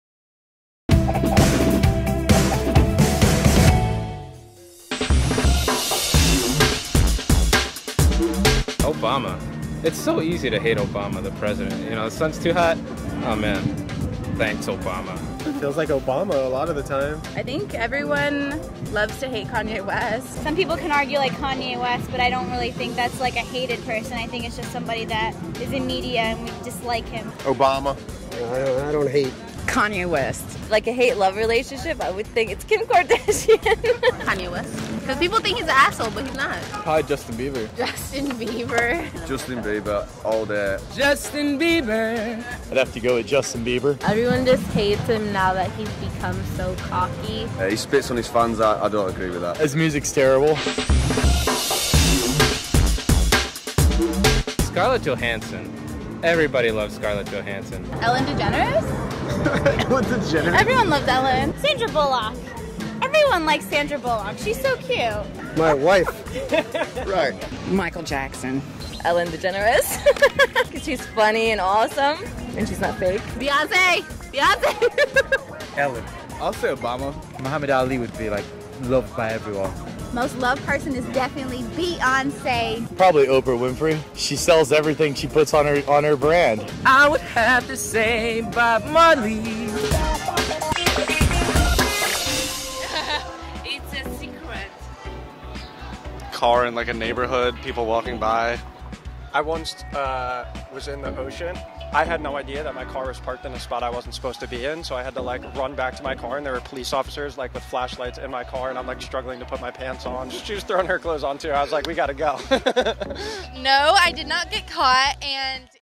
Word on the Street: America's Most Hated and Loved People
Sometimes our society feels like one giant popularity contest. Who is the most hated person in America? And who is the most loved? Playboy hits up people on Los Angeles’s Venice Beach to find out.